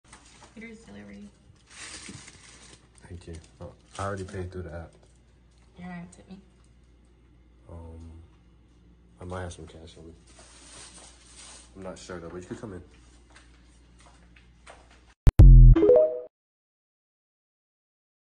I didn’t know Hooters Delivers ‼️😱
Who knew that Hooters delivered ? I sure didn’t. You have to make sure you tip your delivery person ! They’re the ones that handle your food before you get it. Would hate to have a few fries missing lol

Follow us on instagrams and TikTok for more ‼️